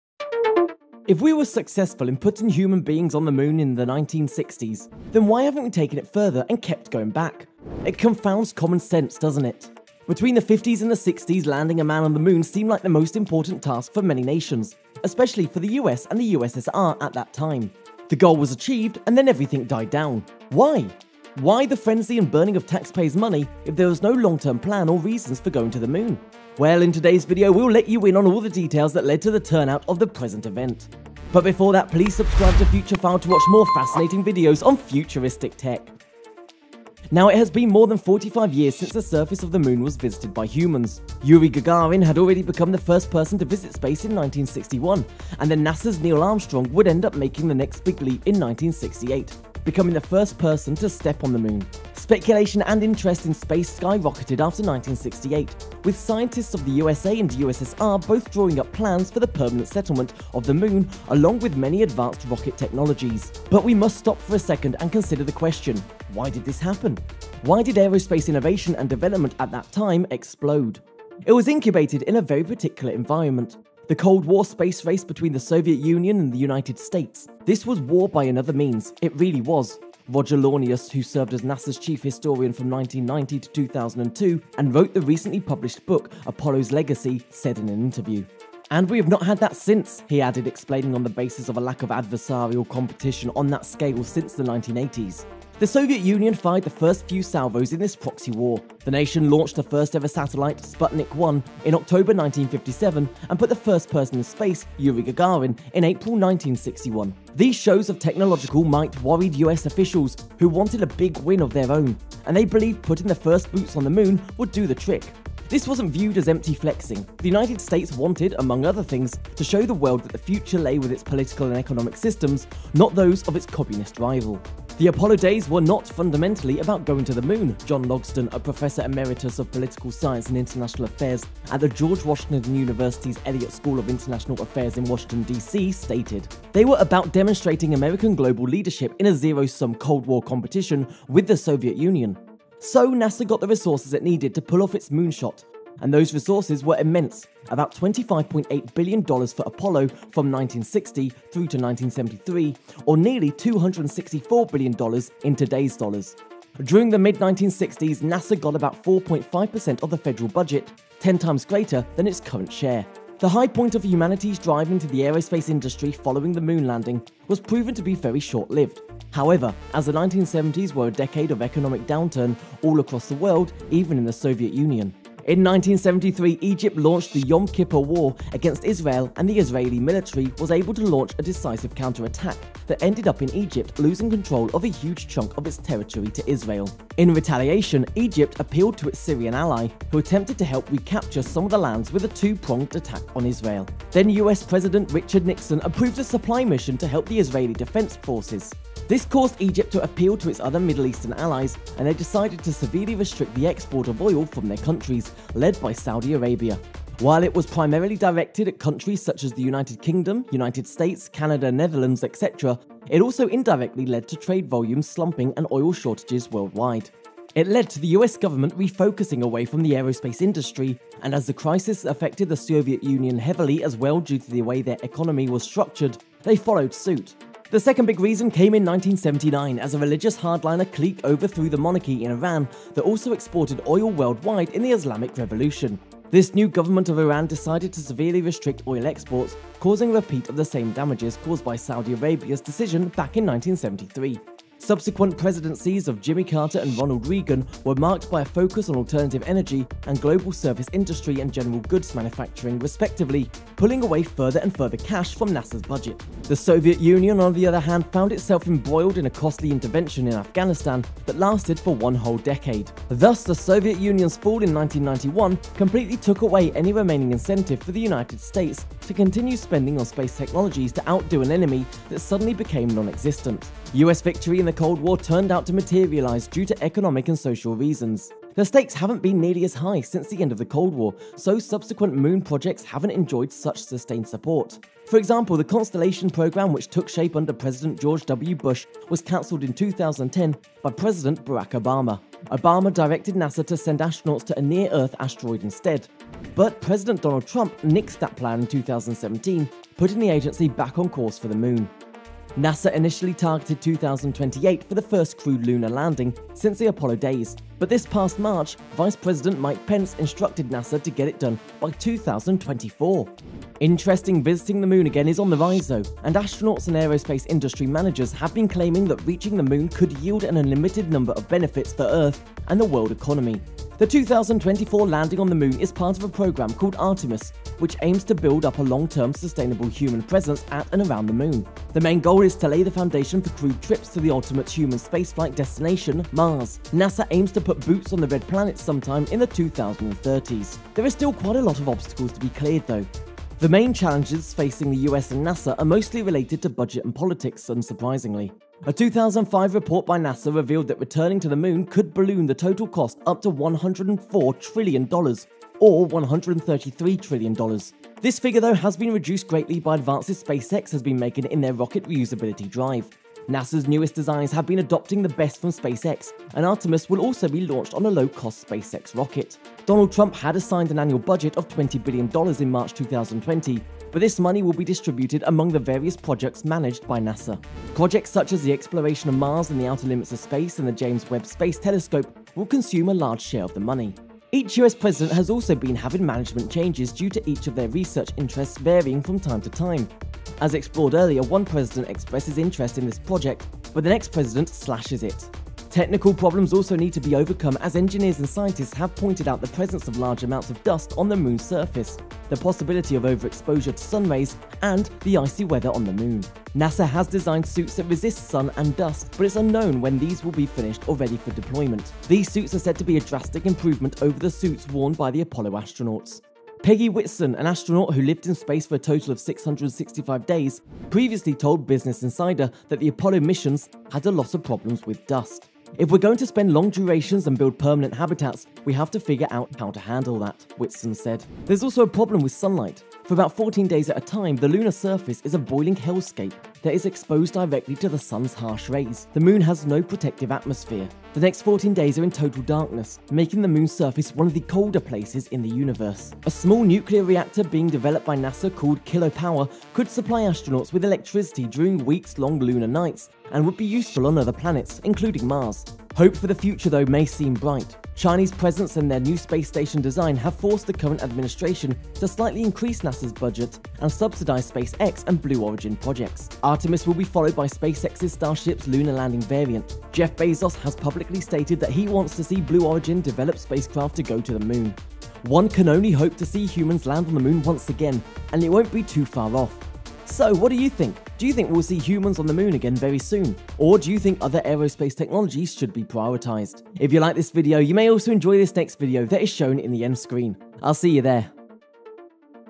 Why Humans Have Not Returned To Moon Since 1972
It's 2021. Why Haven't Humans Gone Back to the Moon Since 1972.
Yuri Gagarin had already become the first person to visit space in 1961, and then NASA’s Neil Armstrong would end up making the next big leap in 1968, becoming the first person to step on the moon.
Speculation and interest in space skyrocketed after 1968, with scientists of the USA and USSR both drawing up plans for the permanent settlement of the moon along with many advanced rocket technologies.
It was incubated in a very particular environment: The Cold War space race between the Soviet Union and the United States.
"The Apollo days were not, fundamentally, about going to the moon," John Logsdon, a professor emeritus of political science and international affairs at The George Washington University's Elliott School of International Affairs in Washington, D.C stated "
So, NASA got the resources it needed to pull off its moon shot.
The high point of humanity’s drive into the aerospace industry following the moon landing was proven to be very short-lived, however, as the 1970s were a decade of economic downturn all across the world, even in the Soviet Union.
The stakes haven't been nearly as high since the end of the Cold War, so subsequent moon projects haven't enjoyed such sustained support.
NASA initially targeted 2028 for the first crewed lunar landing since the Apollo days.
Interest in visiting the moon again is on the rise though, and astronauts and aerospace industry managers have been claiming that reaching the moon could yield an unlimited number of benefits for Earth and the world economy.
The 2024 landing on the moon is part of a program called Artemis, which aims to build up a long-term, sustainable human presence at and around the moon.
A 2005 report by NASA revealed that returning to the Moon could balloon the total cost up to $104 trillion, or $133 trillion.
For about 14 days at a time, the lunar surface is a boiling hellscape that is exposed directly to the sun's harsh rays; the moon has no protective atmosphere.
Chinese presence and their new space station design have forced the current administration to slightly increase NASA’s budget and subsidize SpaceX and Blue Origin projects.
One can only hope to see humans land on the moon once again, and it won’t be too far off.
Do you think we will see humans on the moon again very soon?